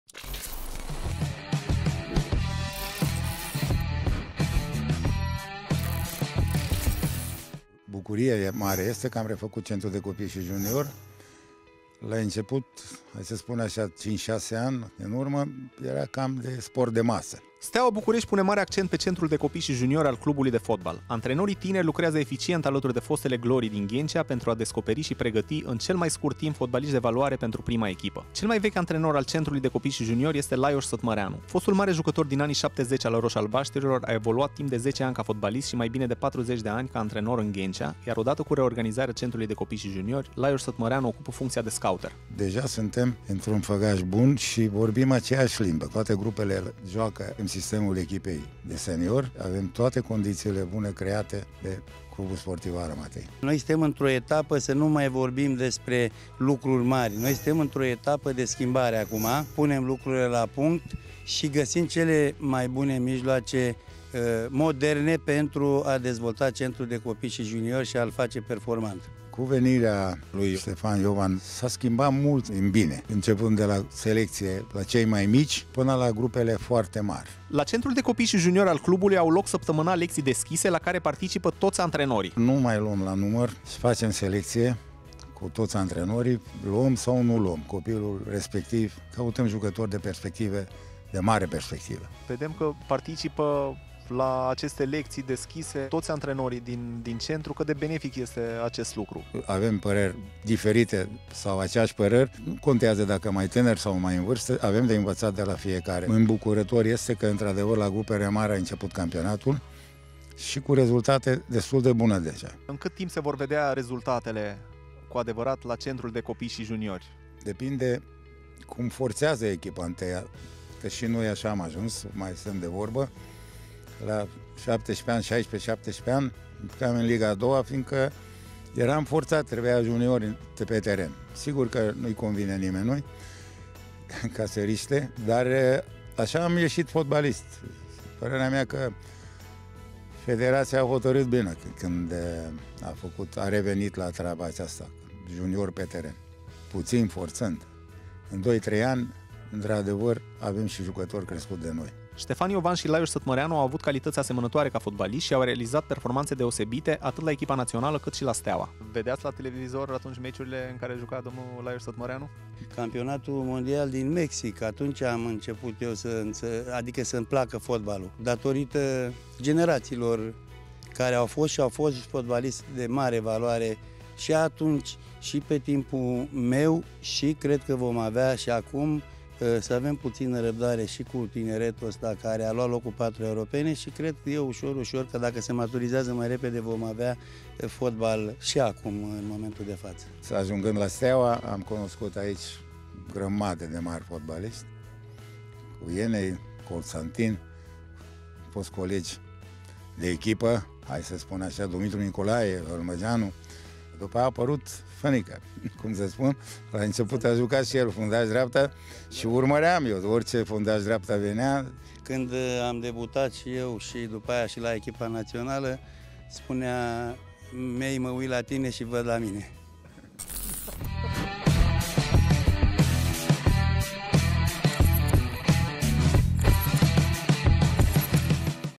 Lajos Sătmăreanu: ”În doi-trei ani vom da jucători la echipa mare”
Steaua București pune mare accent pe centrul de copii și juniori al clubului de fotbal. Antrenorii tineri lucrează eficient alături de fostele glorii din Ghencea pentru a descoperi și pregăti, în cel mai scurt timp, fotbaliști de valoare pentru prima echipă.

Cel mai vechi antrenor al centrului de copii și juniori este Lajos Sătmăreanu. Fost mare jucător în anii 70 al roș-albaștrilor a evoluat timp de zece ani ca fotbalist și mai bine de 40 ca antrenor în Ghencea, iar odată cu reorganizarea centrului de copii și juniori, Lajoș Sătmăreanu ocupă funcția de scouter.